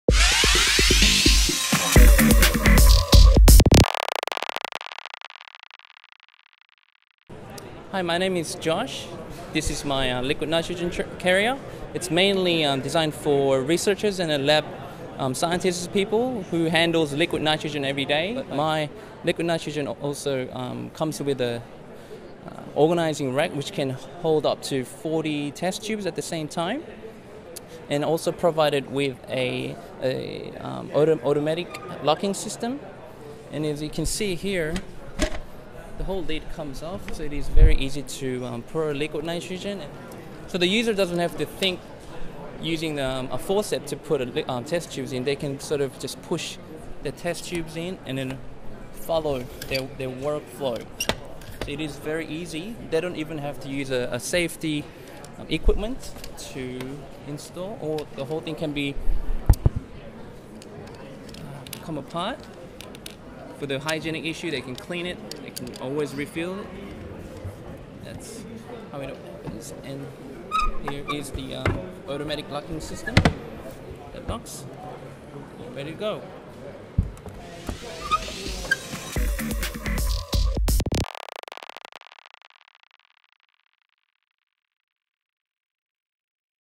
Dyson Awards: Joshua Sunghoon Mun's Liquid Nitrogen Storage
At this year's Australian Design Awards, we asked a selection of the James Dyson award finallists to give us their "elevator pitch" describing their inventions. To find out who won the award,